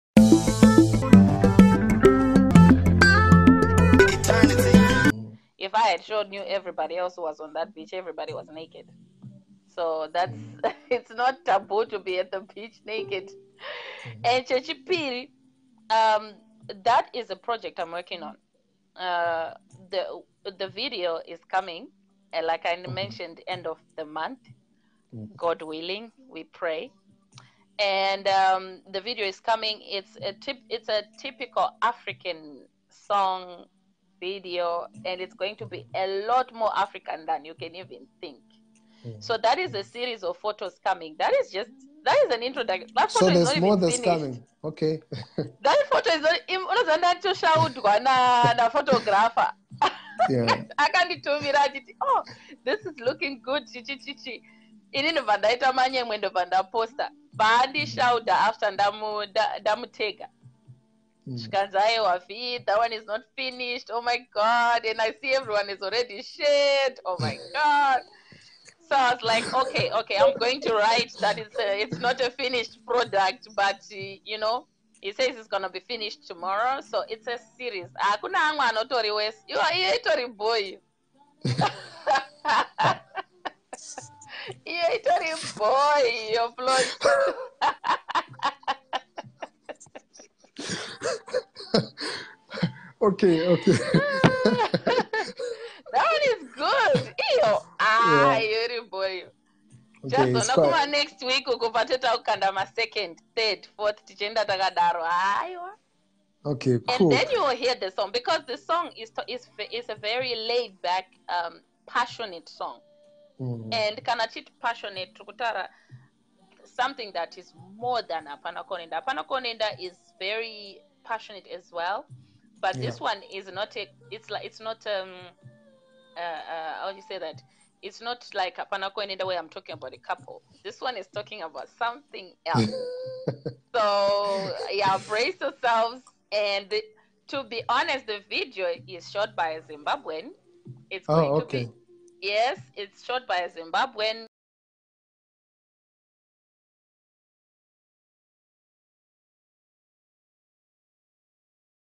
Vimbai Zimuto Speaks to EarGround on Viral pics
Vimbai Zimuto speaks to Plot Mhako. SNIPPET// exclusive interview with EarGround Zw an hour ago. Here is what she said about the picture circulating on Social media//